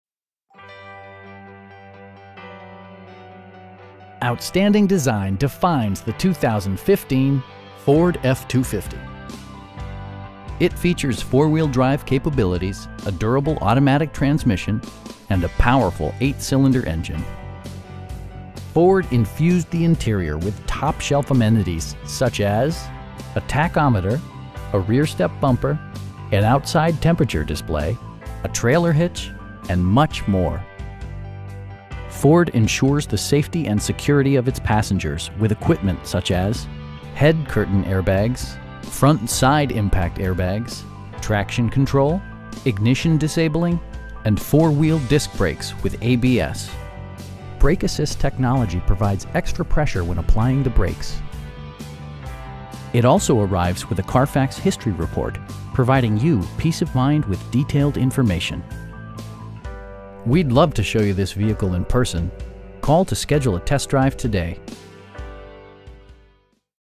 2015 Ford F-250SD Lariat in Elkhart, IN 46514
S&H Motor Sales
51466 State Road 19

Outstanding design defines the 2015 Ford F-250. It features four-wheel drive capabilities, a durable automatic transmission, and a powerful 8 cylinder engine. Ford infused the interior with top shelf amenities, such as: a tachometer, a rear step bumper, an outside temperature display, telescoping steering wheel, an overhead console, a trailer hitch, and much more. Ford ensures the safety and security of its passengers with equipment such as: head curtain airbags, front side impact airbags, traction control, ignition disabling, and 4 wheel disc brakes with ABS. Brake assist technology provides extra pressure when applying the brakes. It also arrives with a Carfax history report, providing you peace of mind with detailed information. We'd love to show you this vehicle in person.